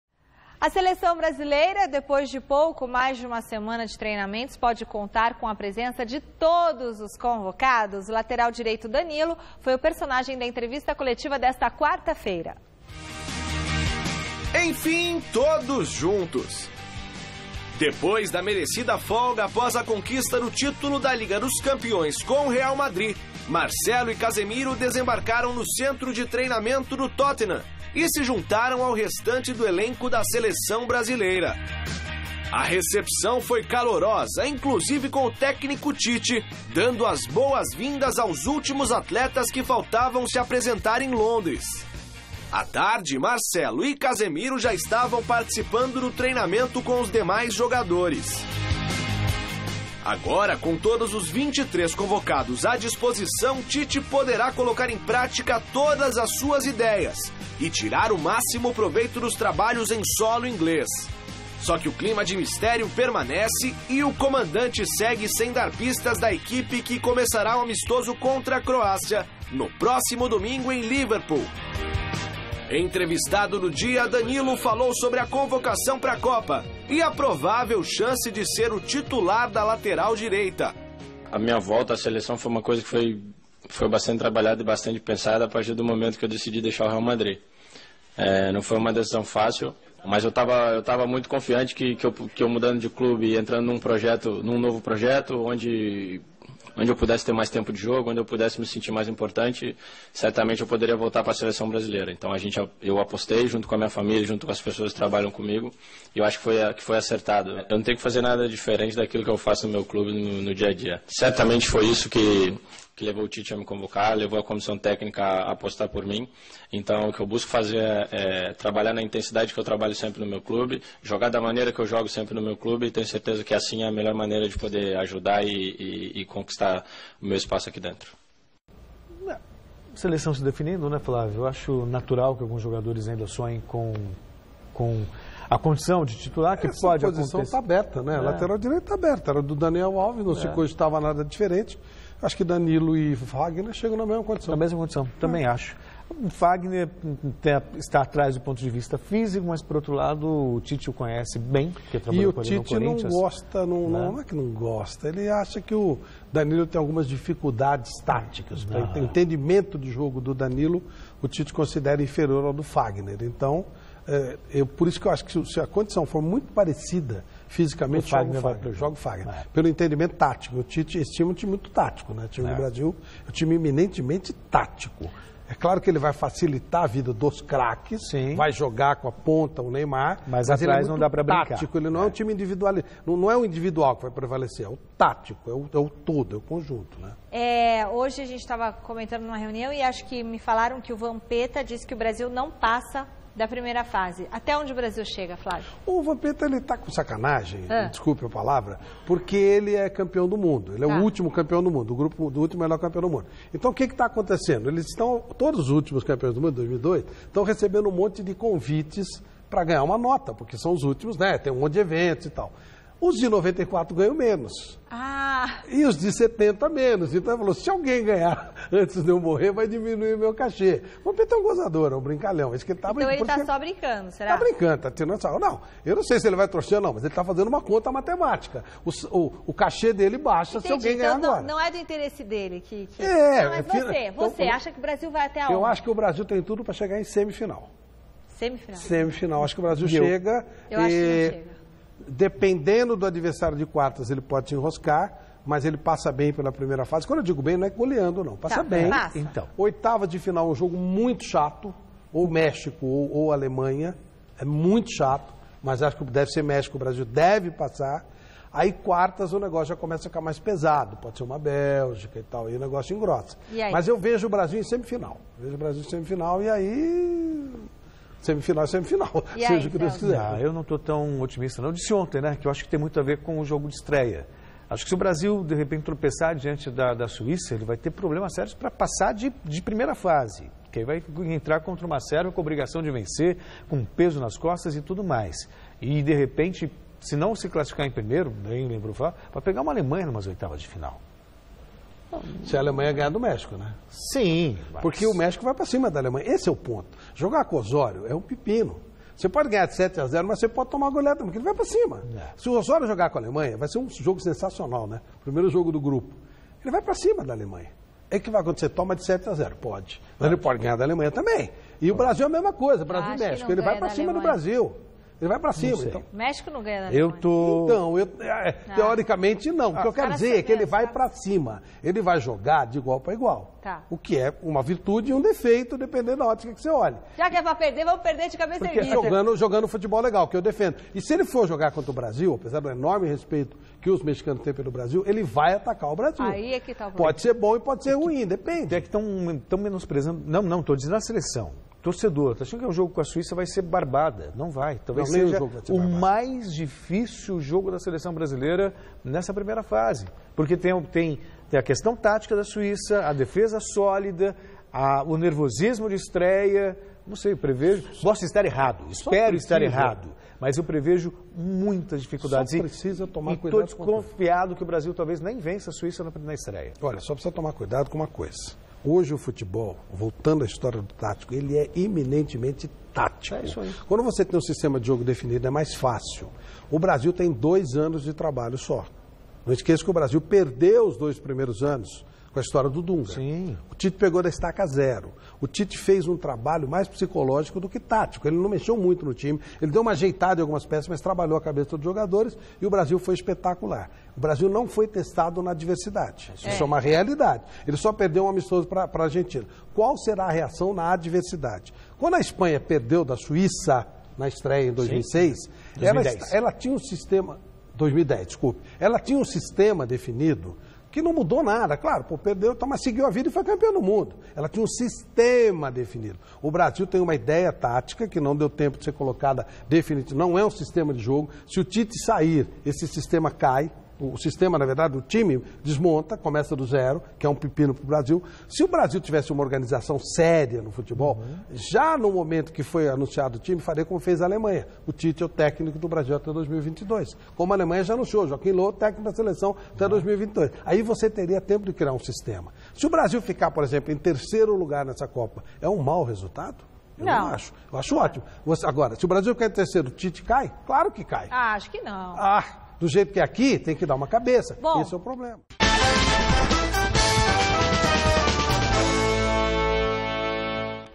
Danilo x Fagner | Mistério no time titular da seleção- Gazeta Esportiva (30/05/18)
Marcelo e Casemiro se apresentam e seleção brasileira está completa para os amistosos antes da Copa